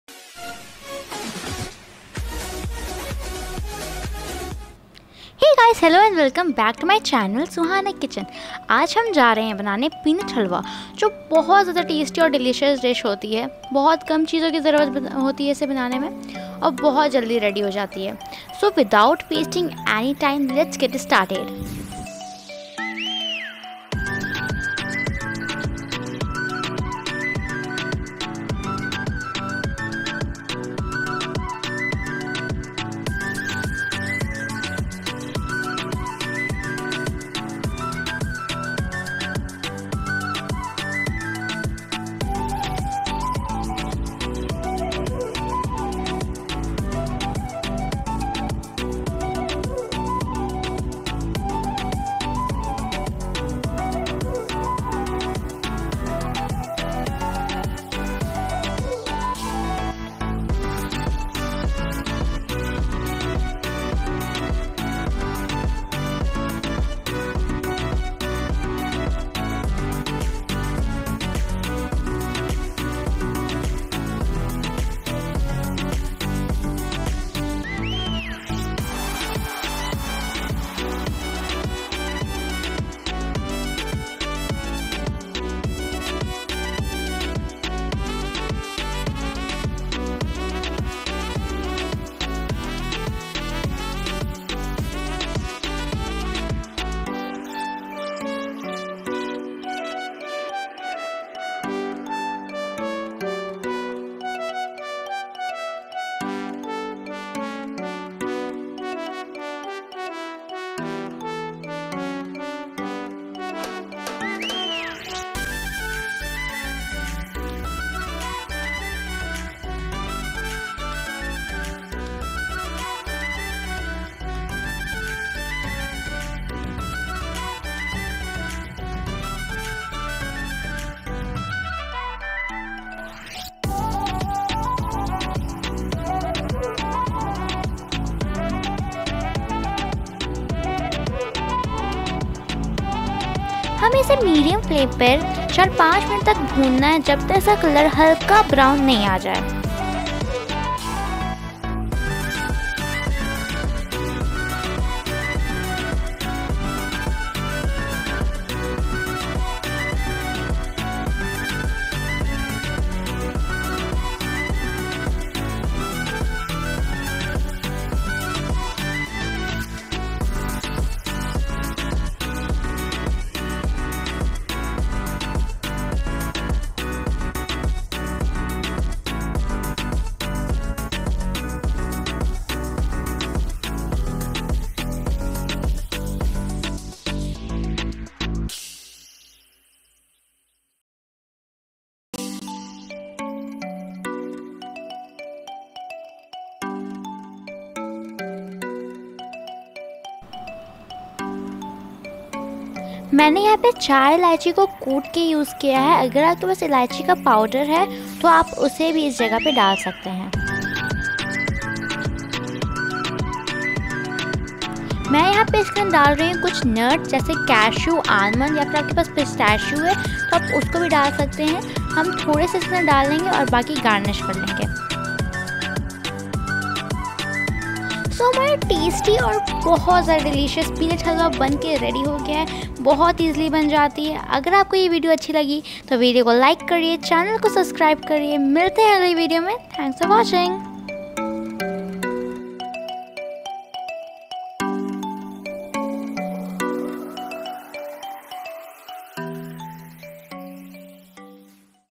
Peanut Halwa recipe | Suhana Kitchen | Hindi
Hey guys,..
Hope you like today's video😊😊
And don't forget totoday's video😊😊
(Homemade Cookies Recipe)

( Cheese Potatoes Balls)

Ingredients.
{1} Over night soaked peanut -500g
{2} Milk - as required tbs.
{3} Ghee - 5 tbs.
{4} Sugar - 1cup.
{5} Cardamom -½ tsp.
{6} Nuts ( Cashew, Almond, Pistachio)

((ㆁᴗㆁ))
Thankyou !...
Take care.

Hashtag...#